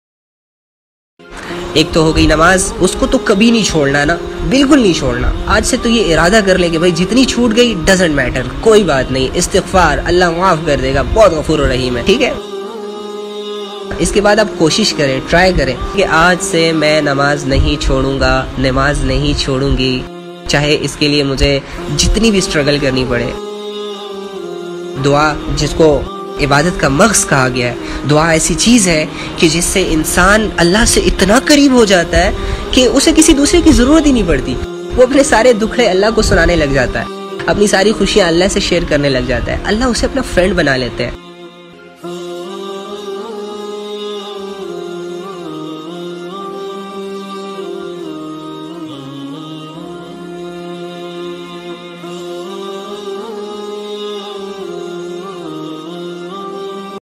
3 bohat he asan kam ha paka irada or paki niyat kar
zindage bohat he masroof tar hoti ja rahi ha day by day laikin jahan ham duniya ko kamaty kamaty bohat sary kam karny parrhty ha waha ham na he akhirat ki wadio main jana hain akhirat k manzil teh karny ha os sa bachna kisi k bus ki bat nai so kindly waha k lia b kuch kary kuch naik amal ma khud ko masroof kary daily basis pay susti na kary .